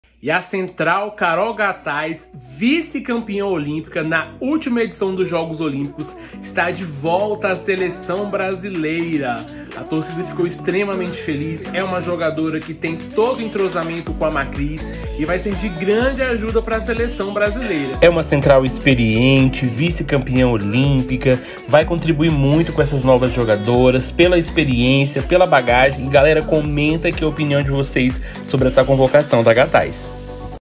Com Carol Gattaz, seleção feminina é convocada - Qual a opinião de vocês?
Volta da vice-campeã olímpica é uma das novidades na lista. Acompanhe!